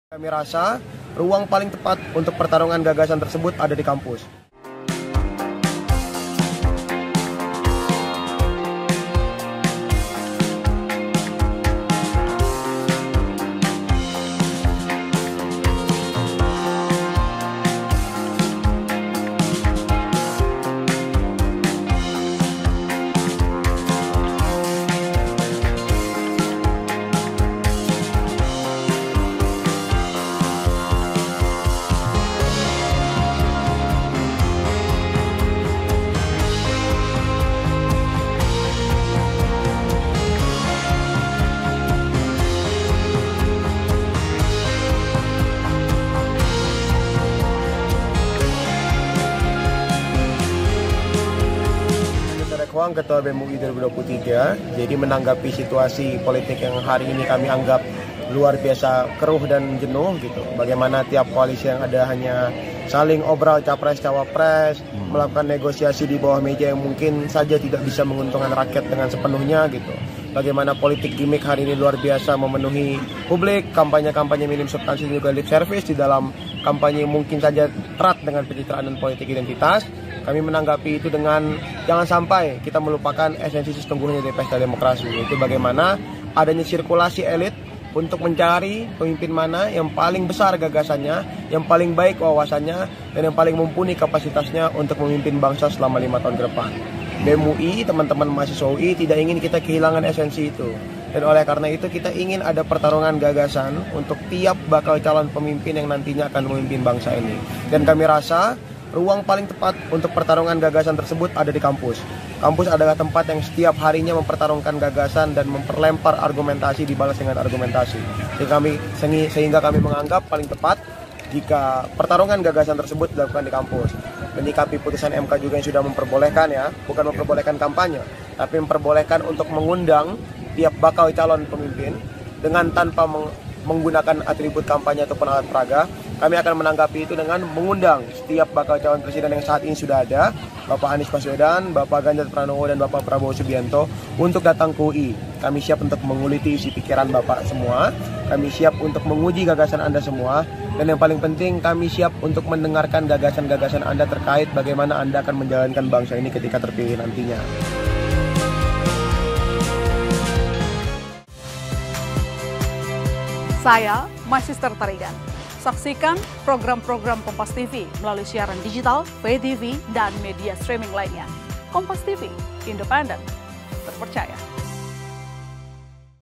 Ungkap Alasan Undang Bakal Capres 2024 Debat di Kampus, BEM UI: Ruang Tepat untuk Adu Gagasan!
DEPOK, KOMPASTV - BEM UI ungkap alasan undang dan tantang tiga Capres Ganjar Pranowo, Prabowo Subianto, dan Anies Baswedan berdebat di kampus.

Ketua BEM UI Melki Sedek Huang mengatakan debat digelar untuk mengembalikan esesnsi pesta demokrasi.

"Kita ngin ada pertarungan gagasan untuk para bakal calon pemimpin," ucap Melki di Depok, Kamis (24/8).

"Ruang paling tepat untuk pertarungan gagasan tersebut ada di kampus," lanjutnya.

Undangan debat tiga Capres ini juga merupakan respons Ketua BEM UI pada putusan MK yang membolehkan hadirnya Capres di kampus.